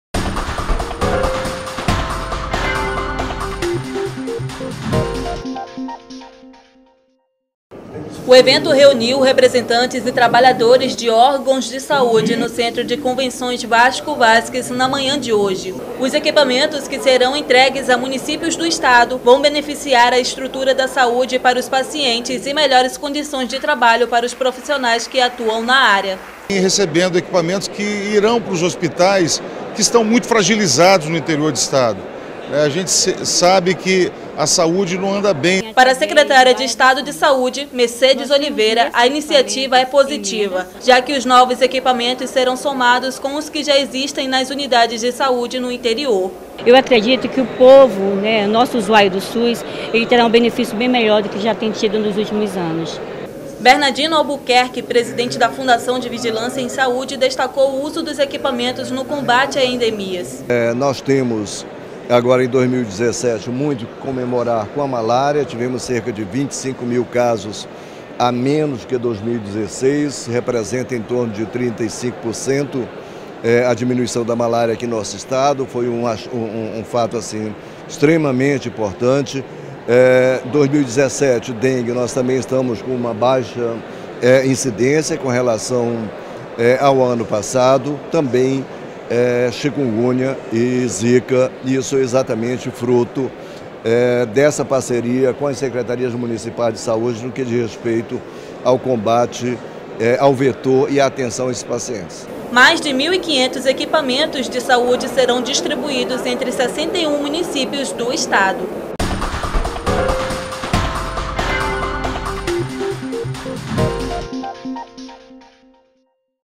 Todos os municípios do estado vão receber equipamentos de saúde
Novos equipamentos de saúde serão entregues a 61 municípios do estado do Amazonas. A medida visa beneficiar a estrutura de saúde e melhores condições de trabalho para os profissionais.